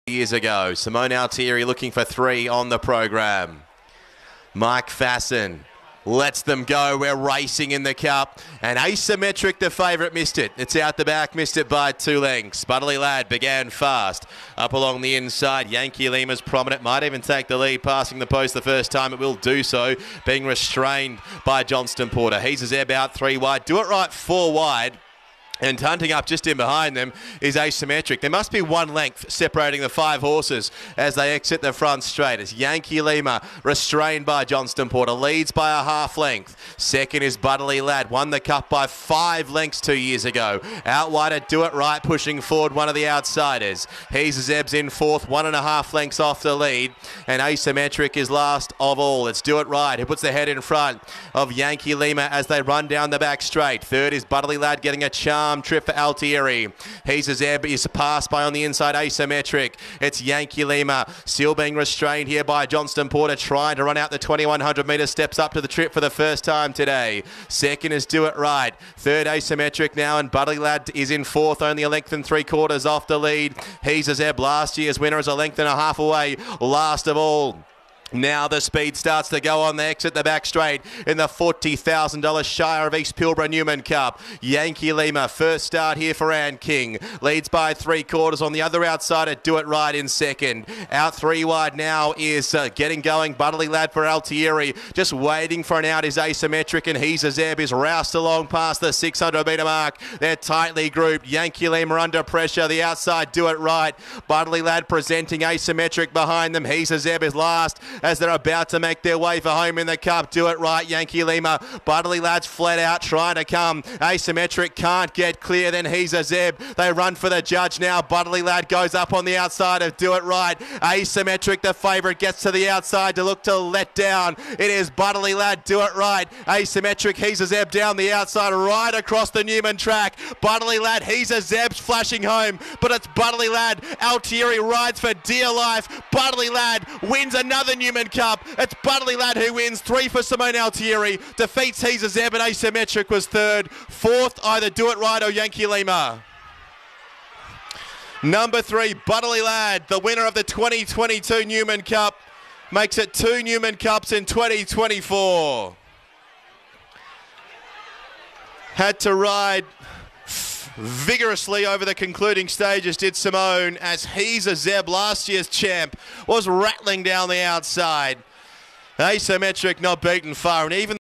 2024 Newman Cup
The Anthony Barton Trained Butterlea Lad Wins The Newman Cup Last Night.